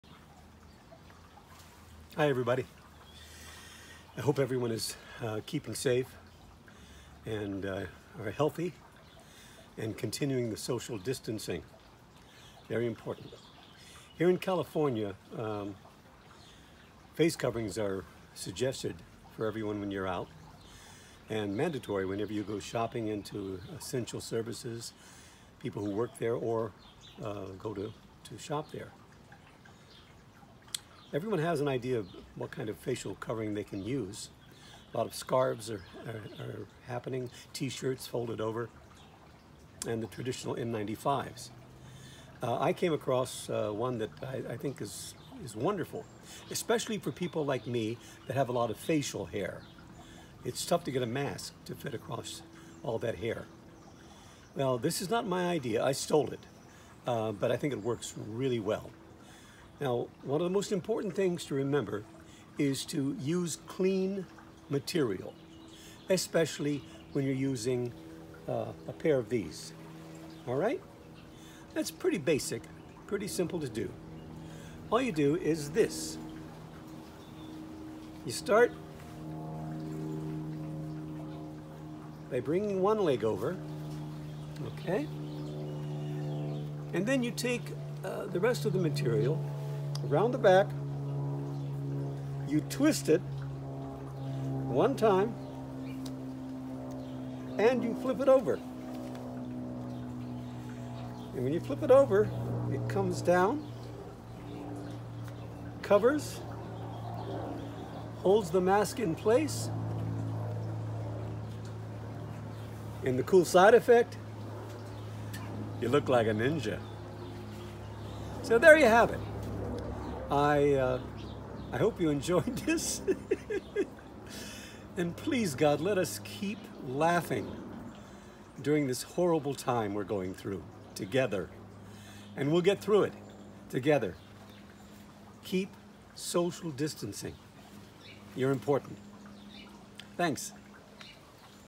A PSA from Paul Eiding
An inventive face covering for all. Especially, men with facial hair. (Not my idea, but too good not to share.)